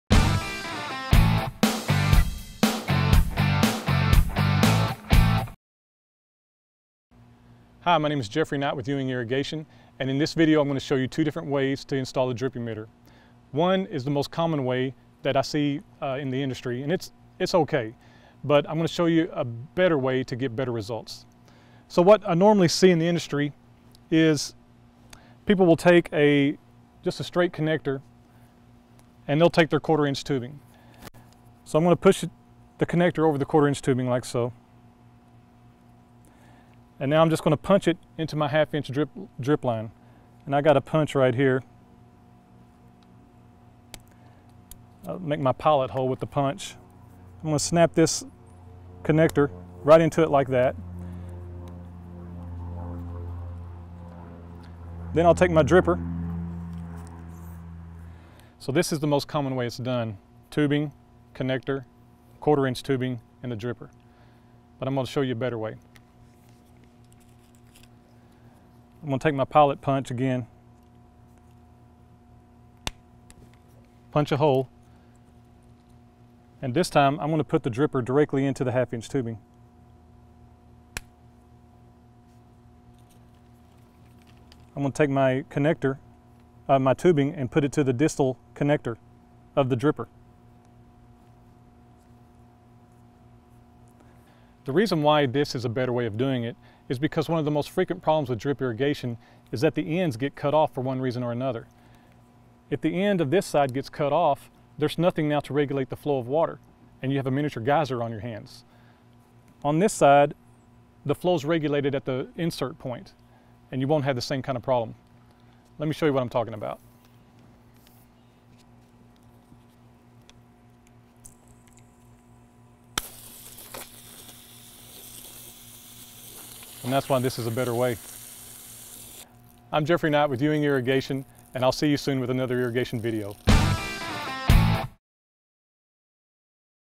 Drip Irrigation Tip - Emitter Placement
A method for installing emitters in drip tubing.